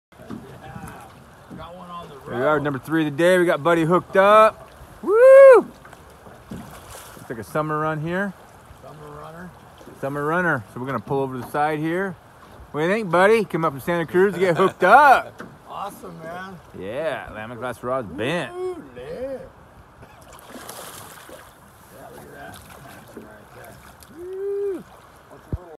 Bobber doggin Steelhead.
Buddy Griggs fighting a summer Steelhead.